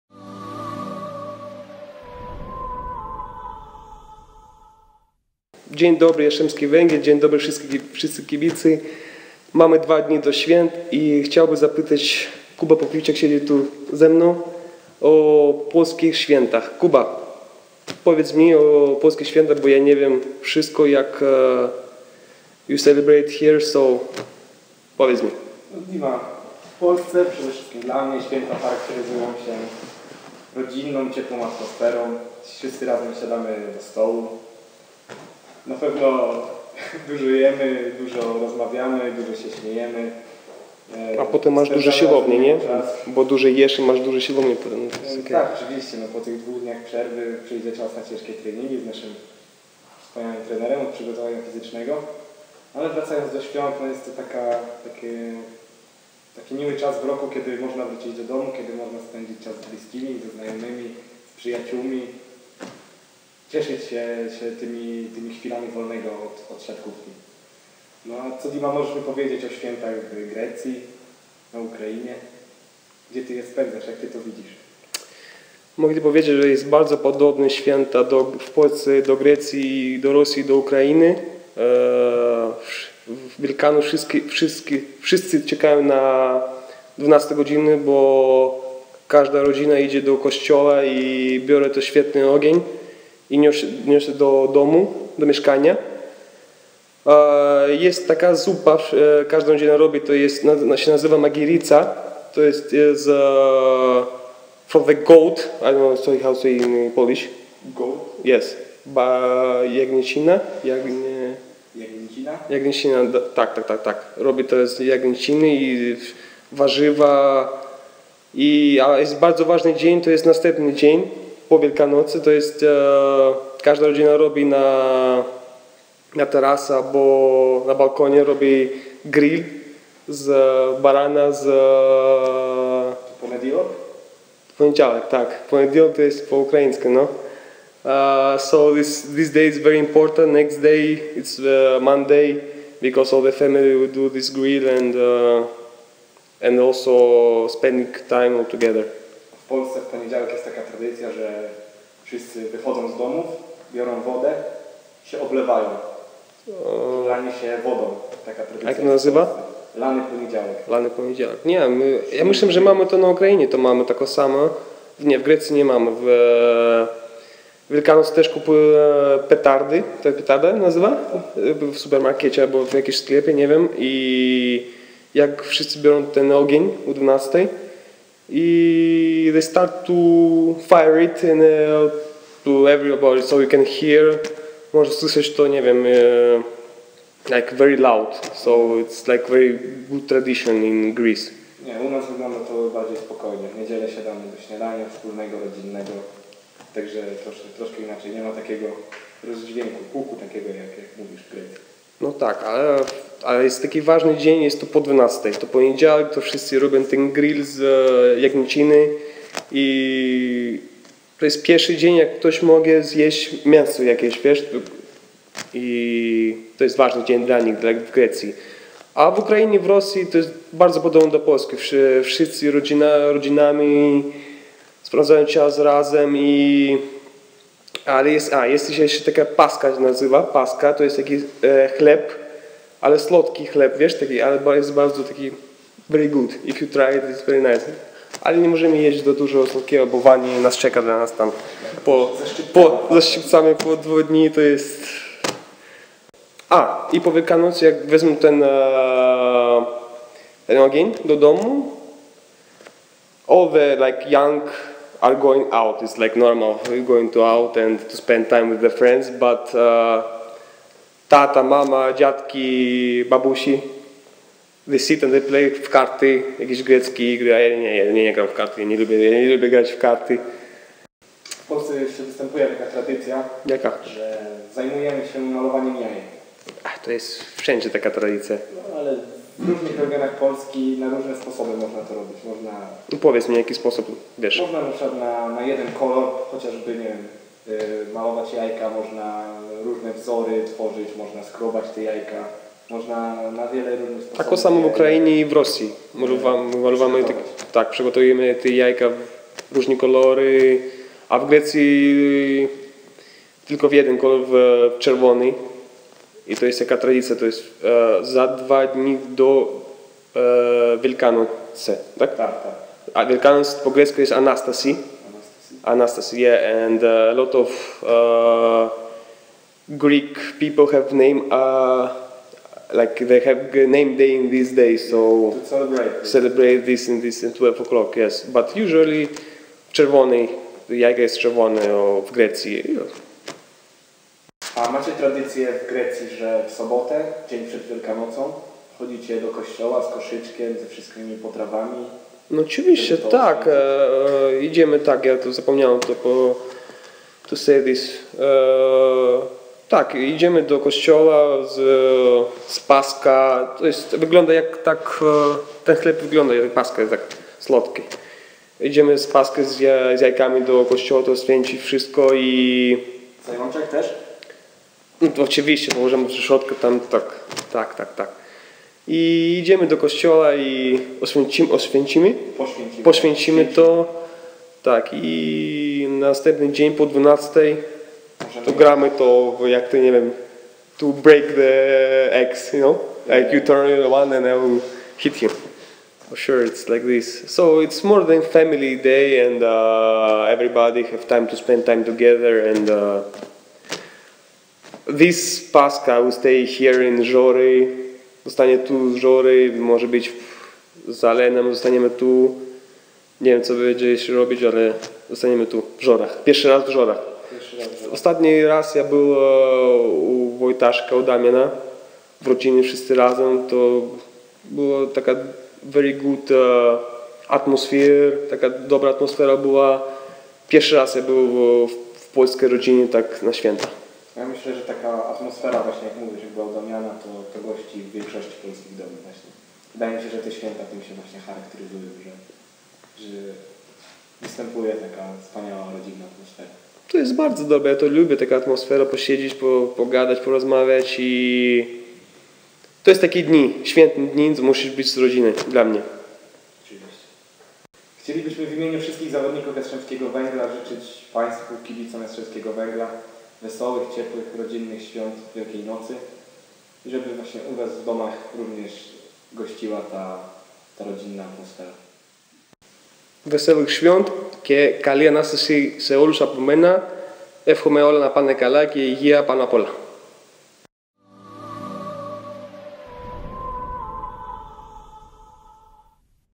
Wielkanocne zwyczaje
Siatkarze Jastrzębskiego Węgla: Jakub Popiwczak i Dmytro Filippov rozmawiają o wielkanocnych zwyczajach w Polsce, Grecji oraz na Ukrainie.